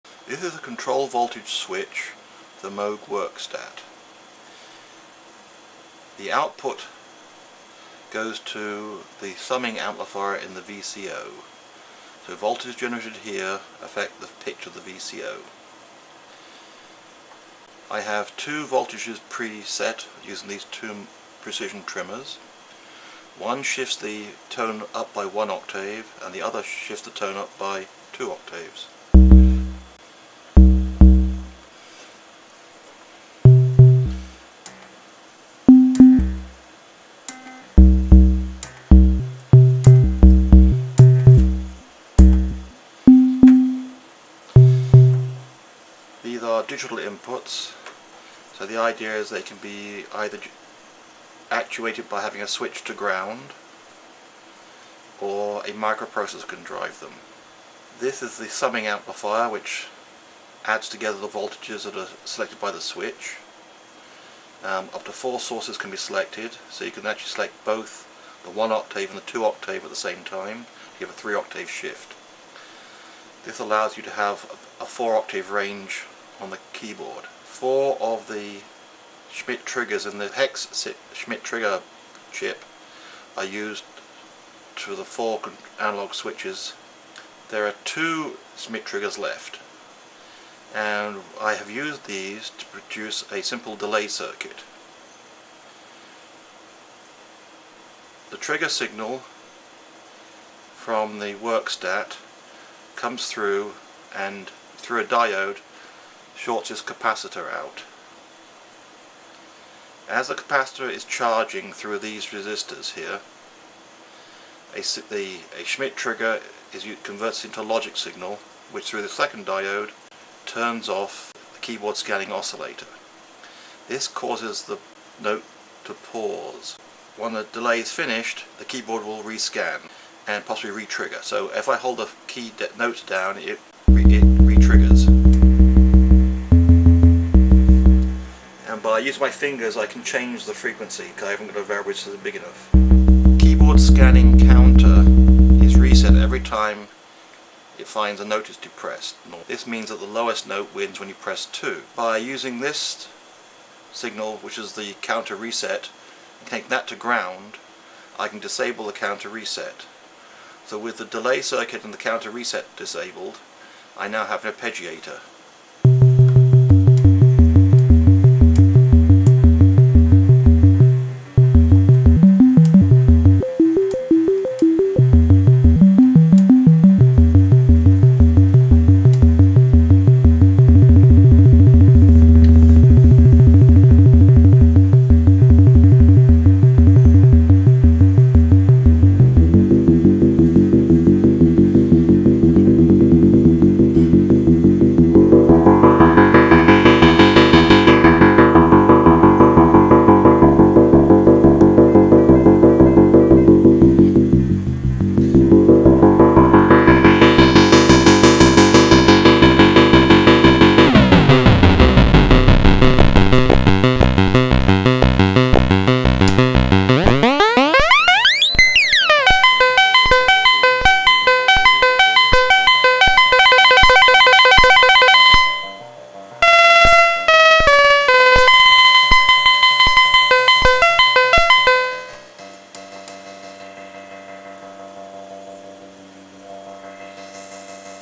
Moog Werkstatt +1, +2 Octave mod and Arpeggiator
This is a video that shows a electronic switch that shifts the VCO pitch by +1, +2 or +3 octaves giving the keyboard a 4 octave range. It also shows a simple delay circuit using left over gates to make an arpeggiator. for more info. Note the Werkstatt sounds were re-recorded so they are not in sync with the video but at least you can hear the octave shifting and the arpeggiator.